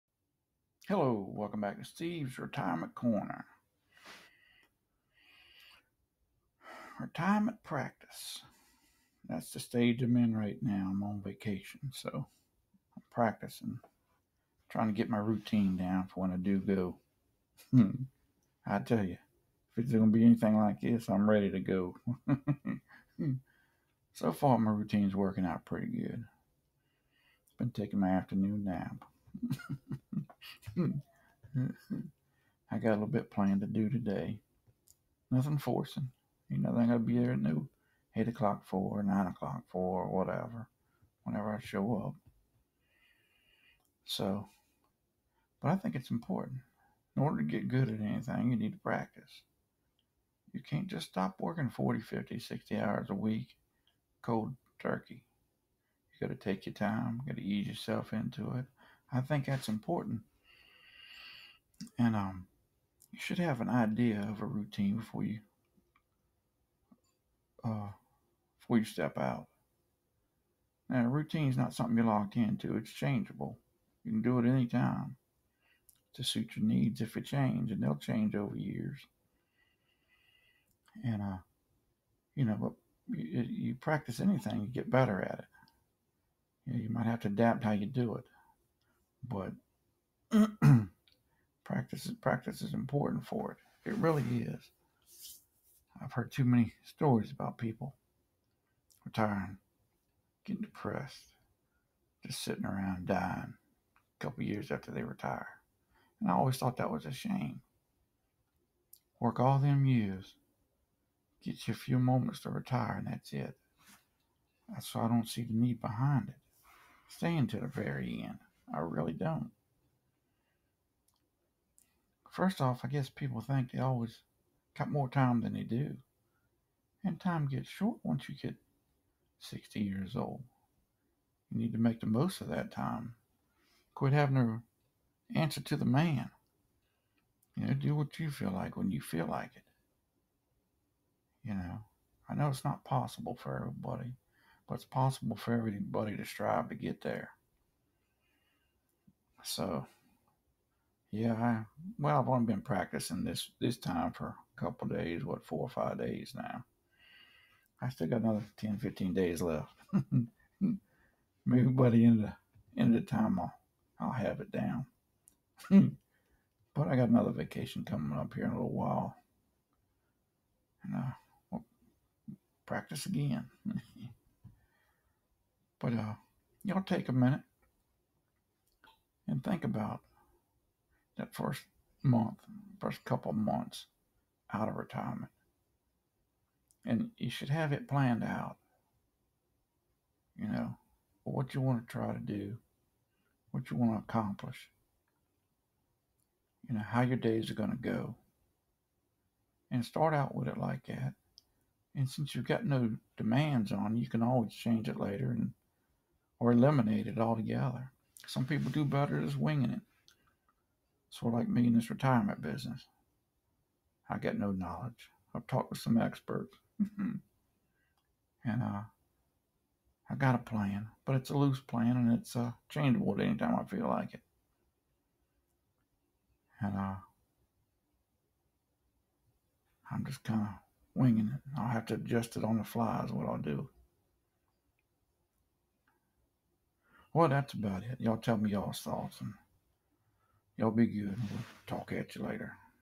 Retirement Practice
To stop doing something 40-50hrs or more a week cold turkey is hard.  I plan on doing a practice retirement on my vacations this year to try and get a feel for life after work. Hopefully by years end I will be able to make the transition fairly easy but time will tell.